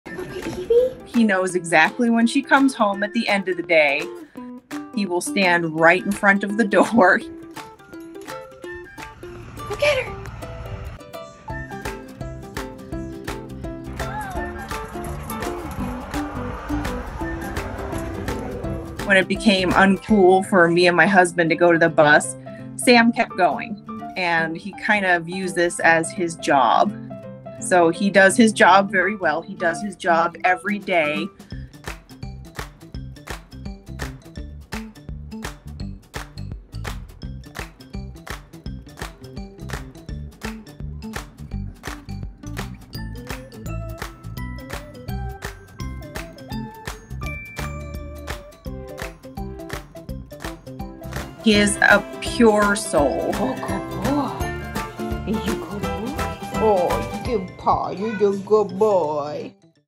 Golden Brings His Sister To The School Bus Every Day | The Dodo
So pure 🤍